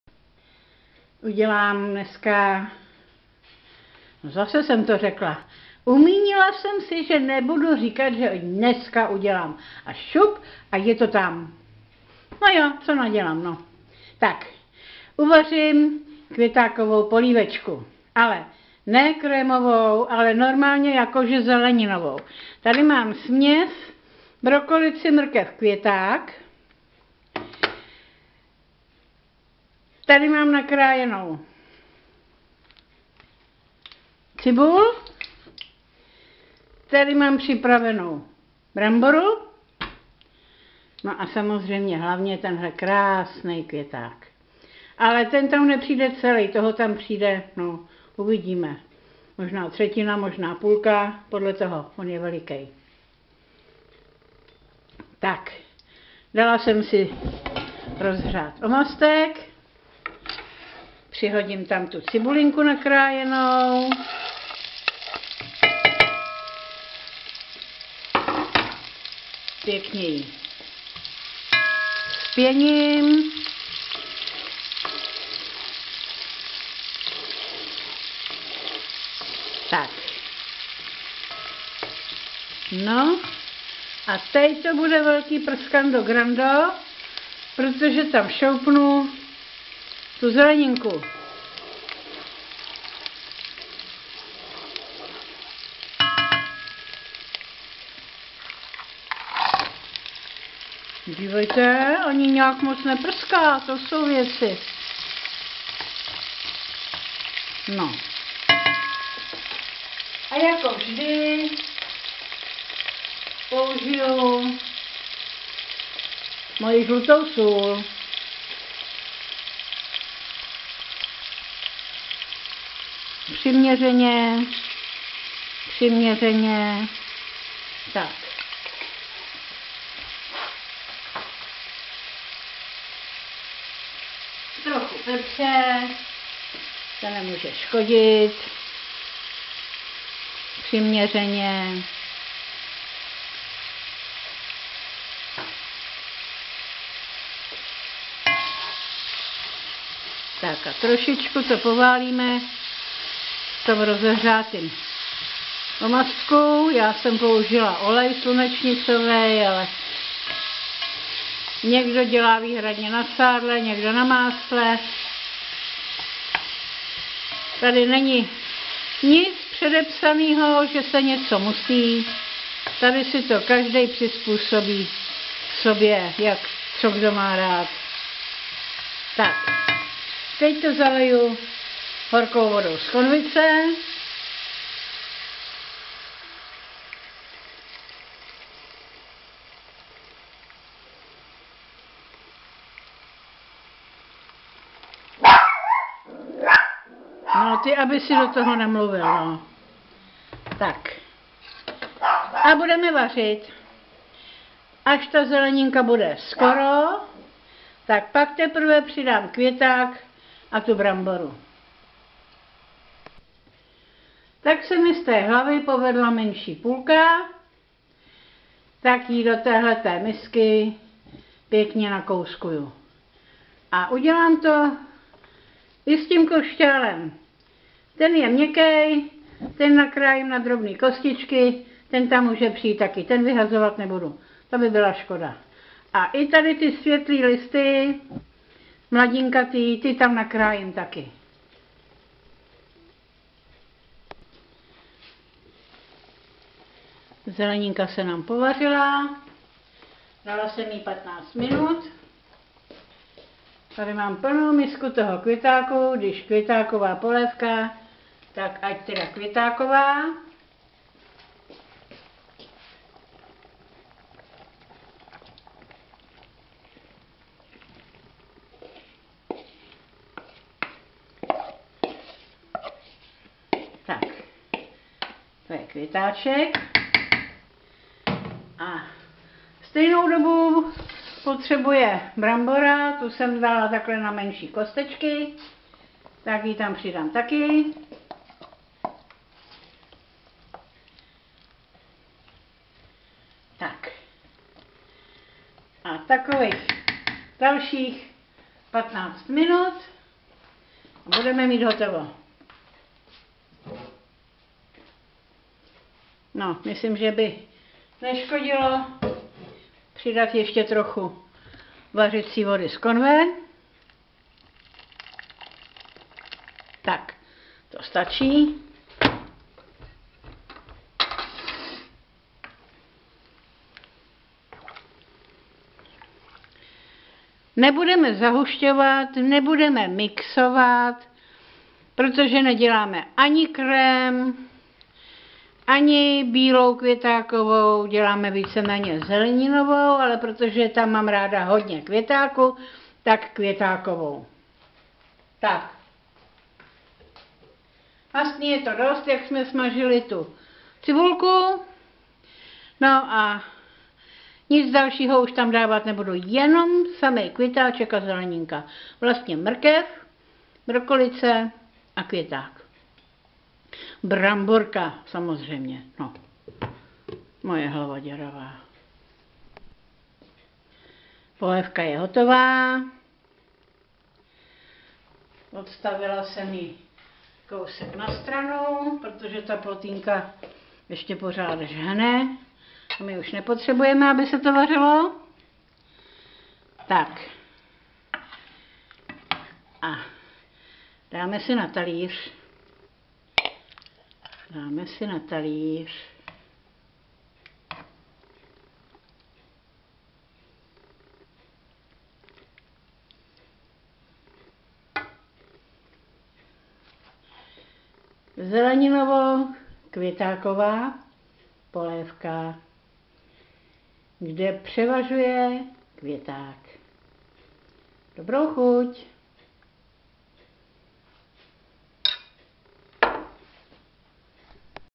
Květáková polévka
S mrkvičkou, brokolicí a bramborem .